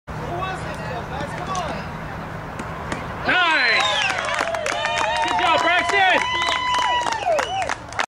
7-12-13 - It's the 2nd strikeout in a row for Braxton Shelton
Fort Morgan Mustangs youth baseball vs. Warrior Fire in the 2nd game of pool play in the 10 and under AA USSSA state tournament from Creekside Park in Wheat Ridge, Colorado on July 12, 2013. Mustangs defeated the Fire, 10-0.